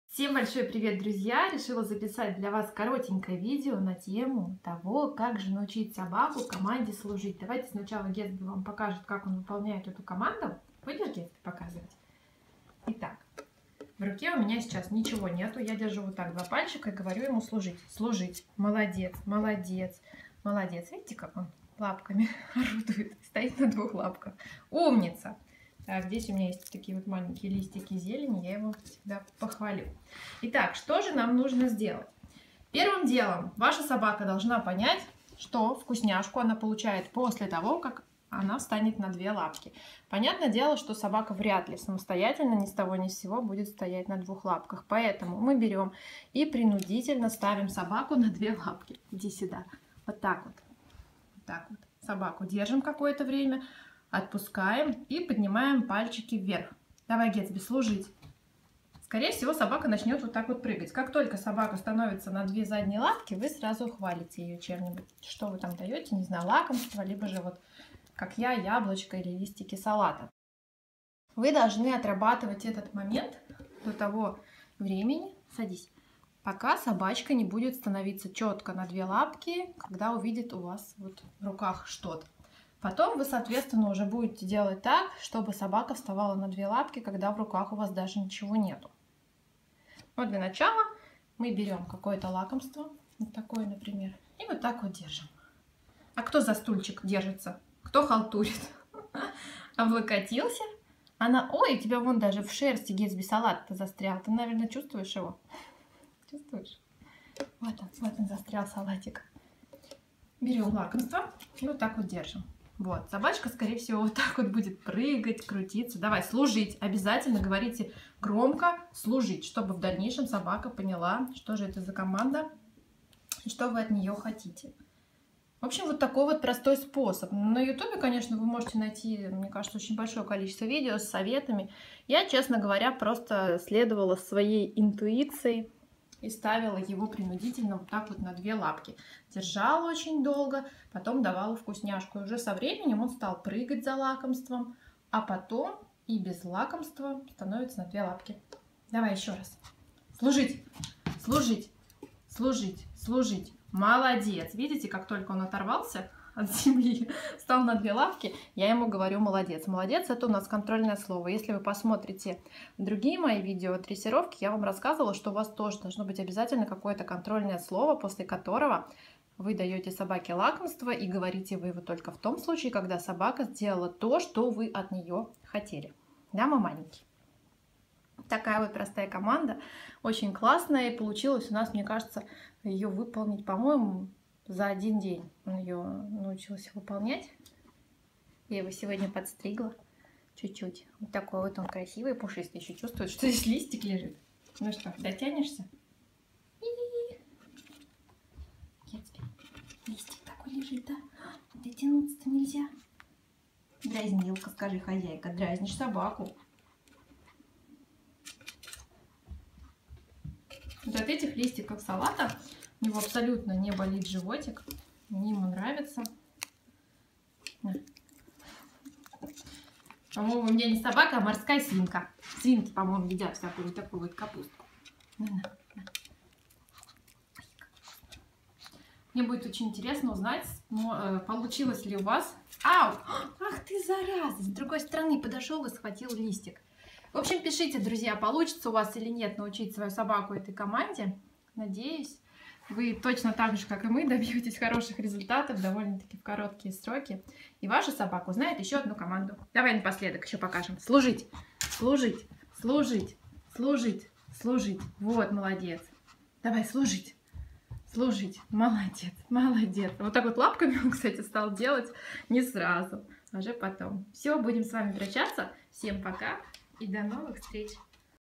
КОМАНДА СЛУЖИТЬ .ДРЕССИРОВКА ЩЕНКА.
Всем привет) В этом видео я расскажу Вам как очень просто научить собаку стоять на двух лапах -выполнять команду Служить.

ВСЕ ВИДЕО О ДРЕССИРОВКЕ :

1.ОСНОВЫ ДРЕССИРОВКИ .КОМАНДА МЕСТО

2.КОМАНДА СИДЕТЬ

3.КОМАНДА ТОННЕЛЬ+DIY (как сделать его из коробки)